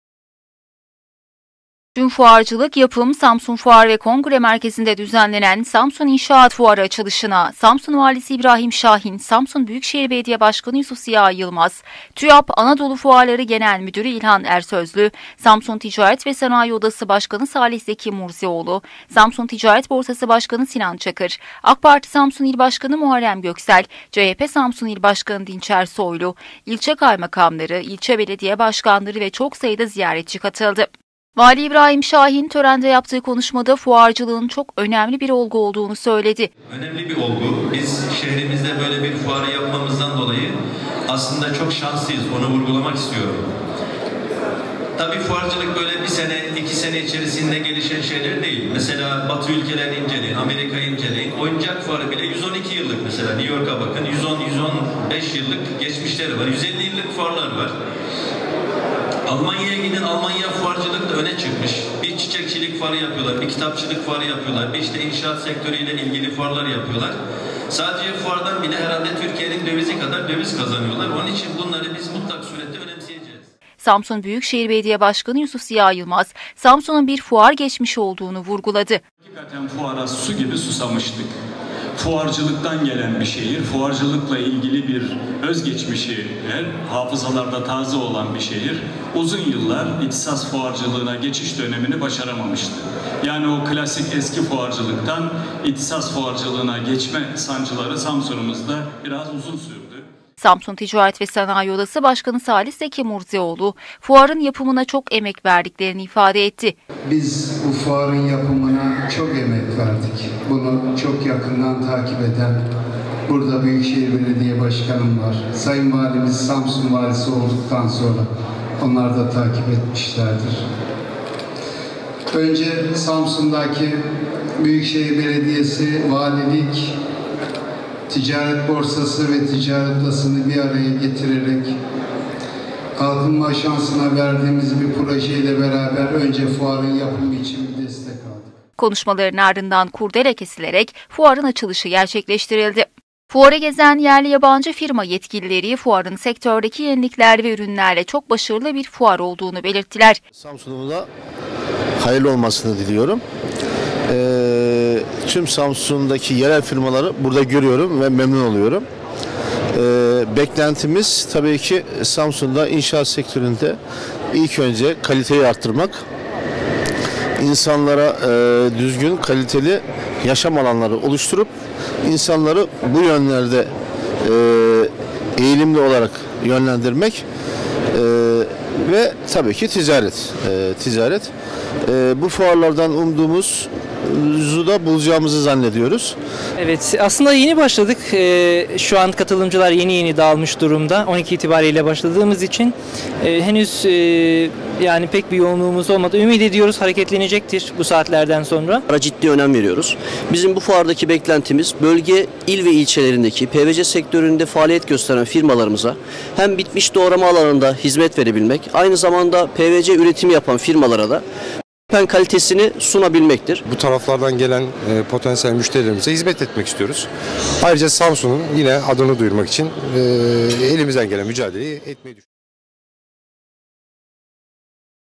SAMSUN İNŞAAT FUARI AÇILDI
Tüm Fuarcılık Yapım A.Ş(TÜYAP) tarafından düzenlenen ‘Samsun İnşaat Fuarı 2015’ düzenlenen törenle kapılarını ziyaretçilerine açtı.